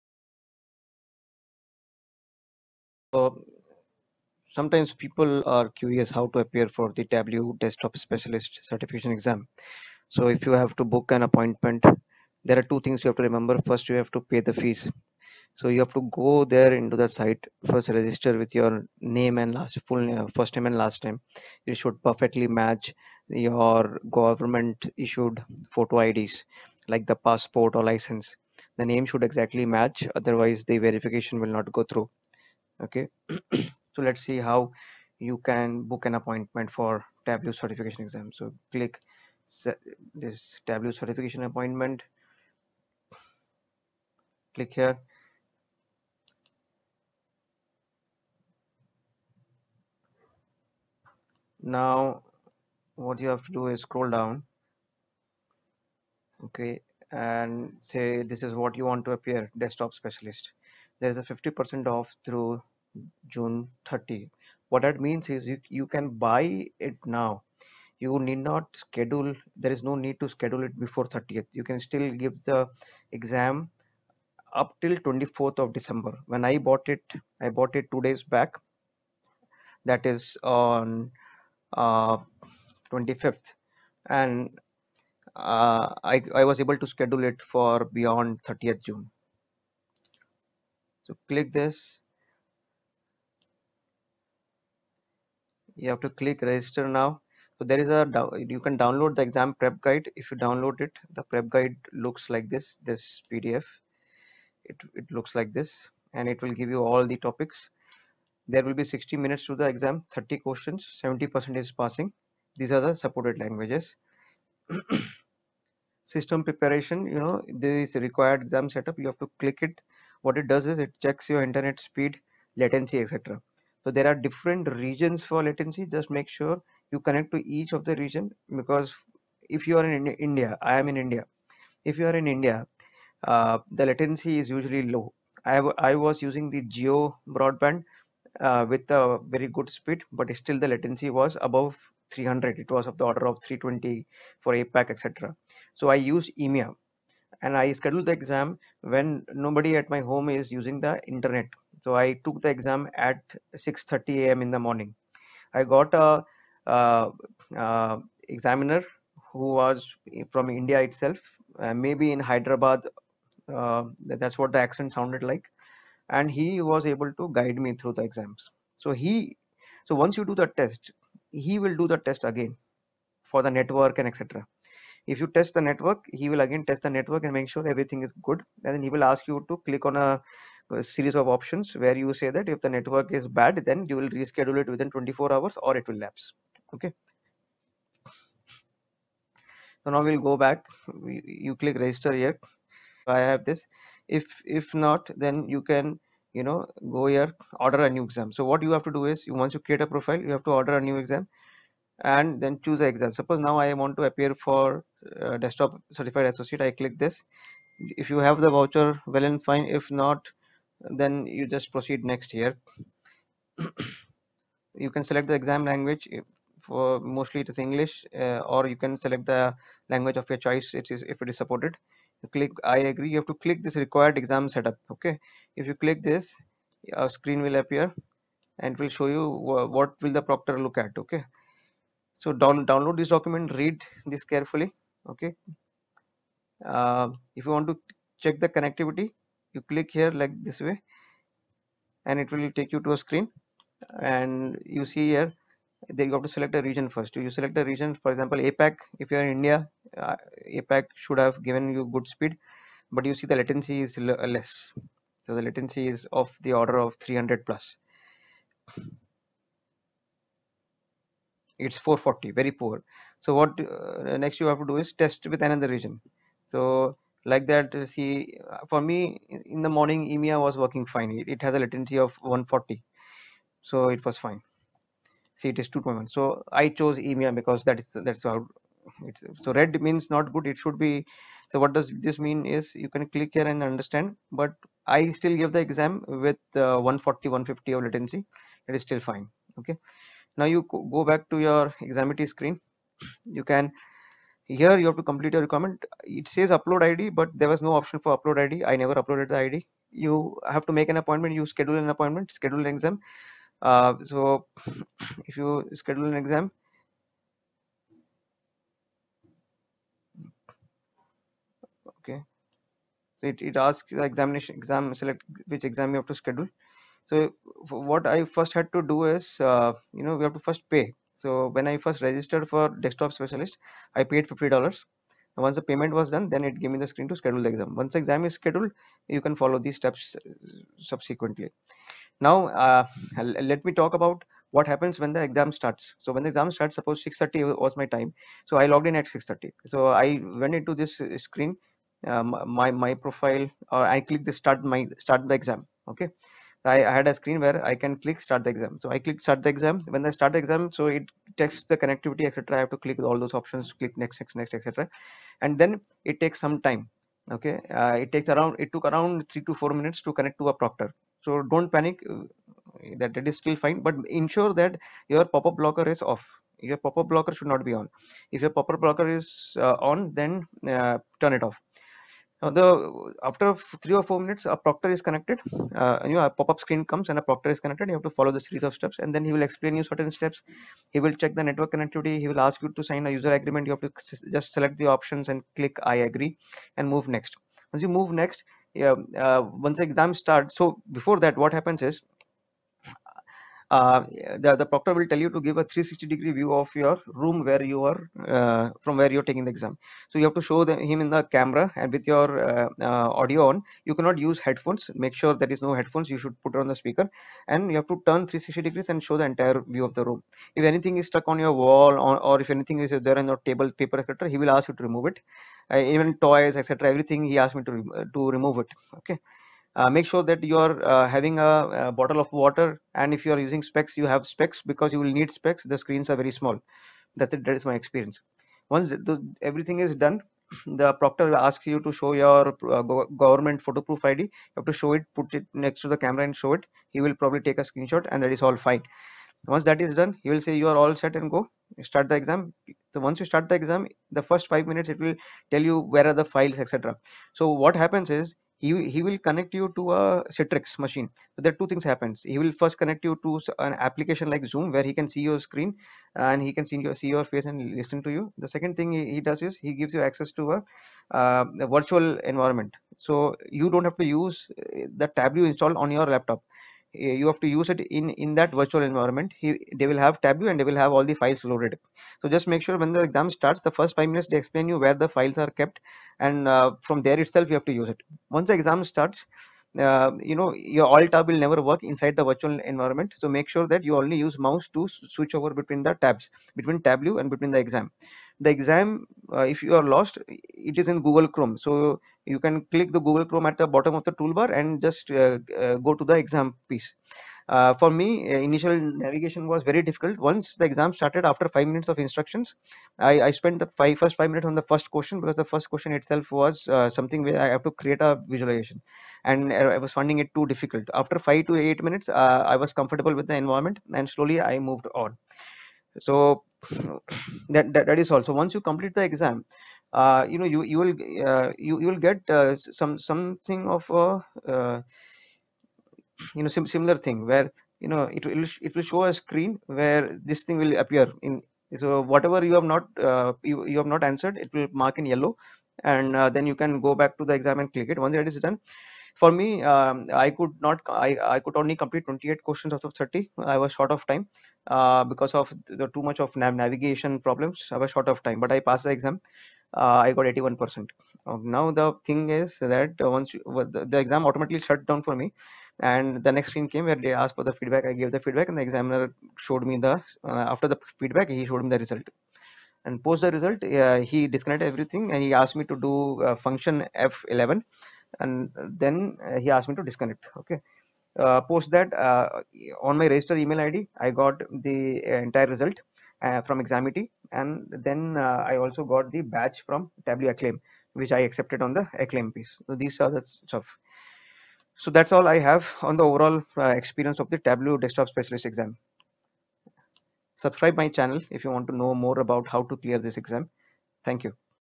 Tableau Desktop Specialist Exam Experience
The channel owner is sharing his experience with taking an appointment for Tableau Desktop Specialist certification and explains the overall exam experience.